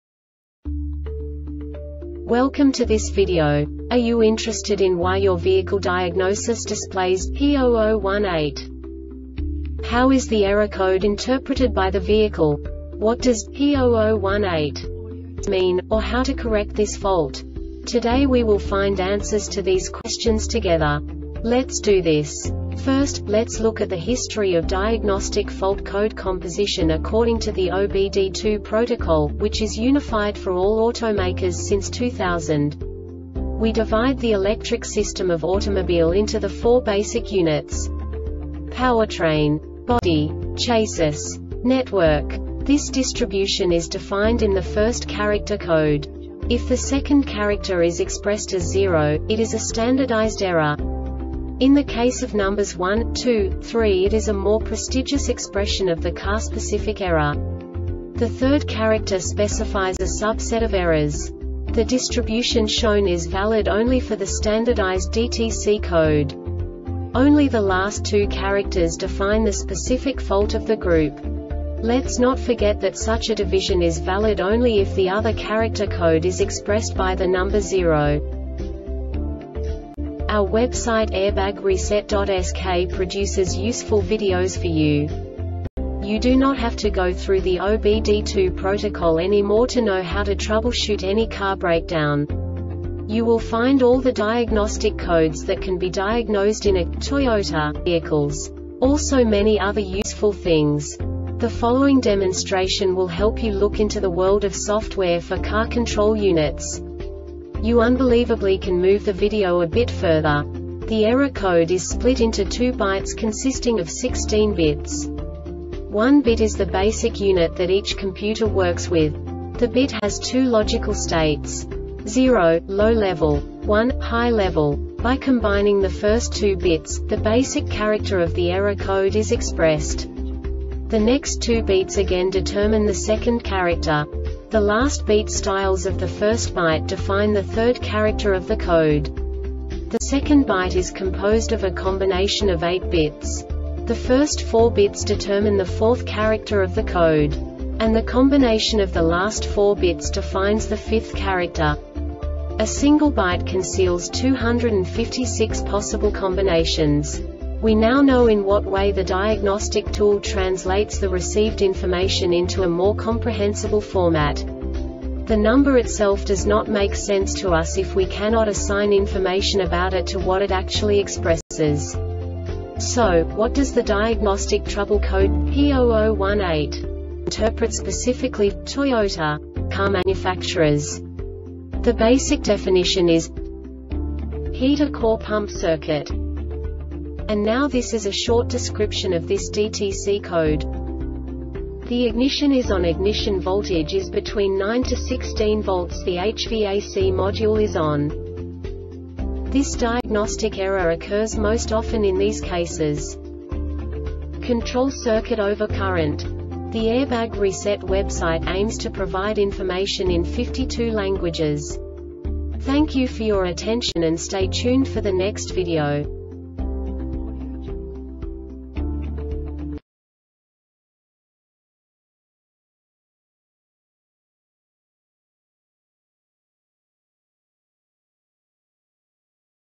DTC Toyota P0018 Short Explanation
Contents:
0:21 Basic DTC analysis according to OBD2 protocol standard.
1:48 Insight into programming
2:58 A diagnostically understandable description of the Diagnostic Trouble Code
3:05 Basic error definition

Make:
Toyota
Code:
P0018
Definition:
Heater Core Pump Circuit
Description: